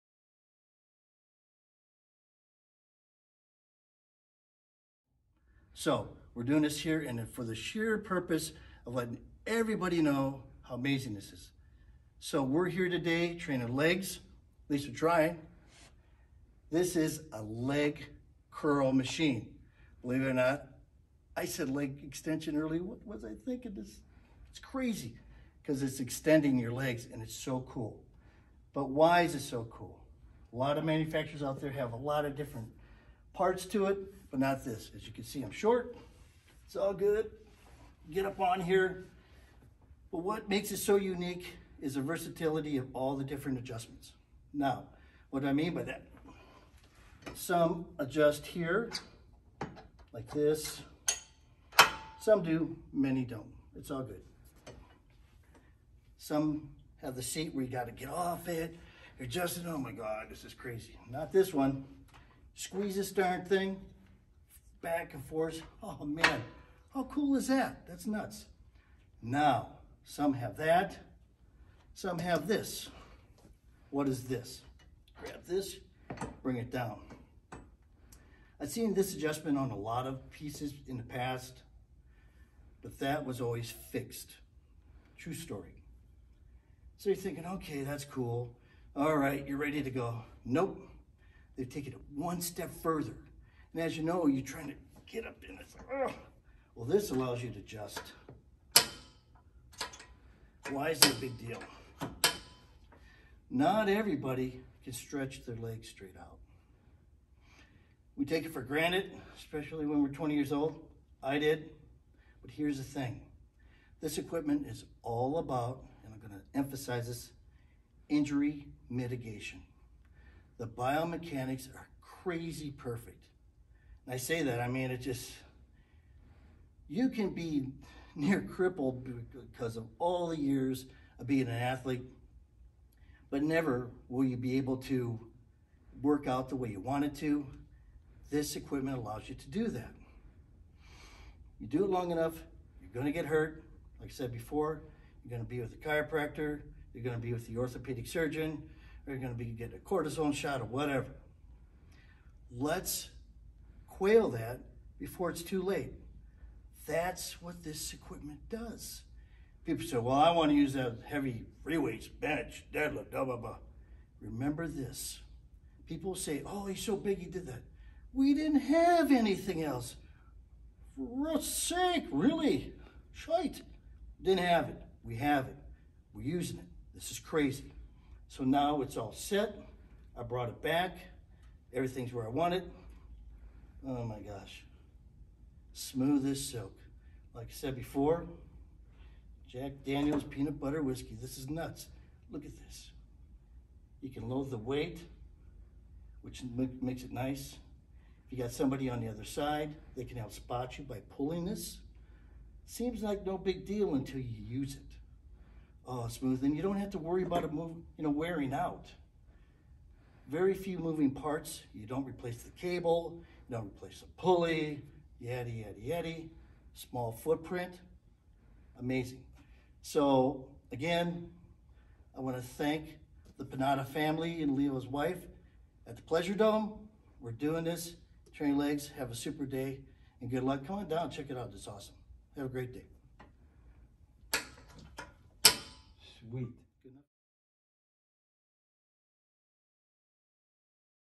Panatta Freeweight Seated Leg Curling Machine Demonstration | Panatta Seated Leg Curl 1MTH083
Former bodybuilder Randy Heim describes the Panatta Seated Leg Curling Machine.  If you're looking to add strength and mass to your hamstrings without harming your tendons or joints then this is the machine for you.

The ergonomic fit is unbelievable.  The seat has a gas-assisted backrest which is adjustable in height.  Counterweights help to reset the empty weight of exercise and the levers to adjust and control the position of your legs are intuitive and easy to operate.

If you are interested in the Panatta Seated Leg Curling machine, check it out on our website.  Available for purchase in-store or online: